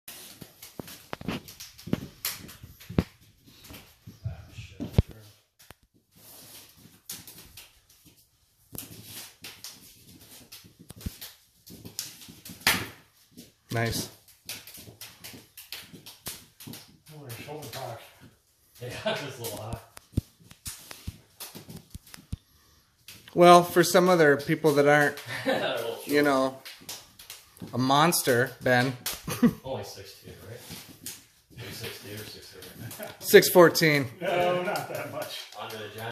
More intense and focused paksau
About one in every ten paksau should have more explosion and intent in the hubad lubad drill.